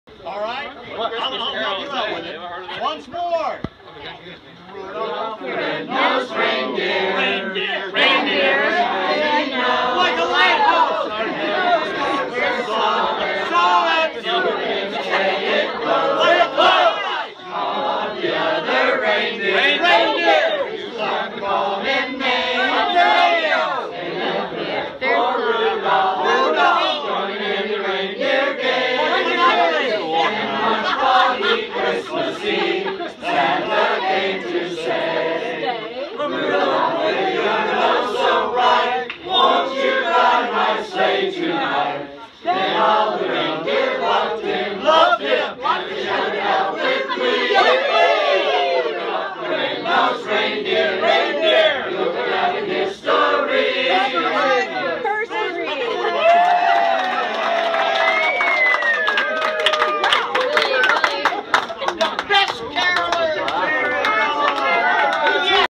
Christmas Carolling In Hopetown, Elbow Cay, Abaco Bahamas
Celebrating Christmas In Hopetown is very merry! Hopetown on Elbow Cay in Abaco is a very social town. The carolers strolled the golf cart sized streets stopping to sing to businesses and homes. This video is outside Wine Down Sip Sip, a common hangout on the island.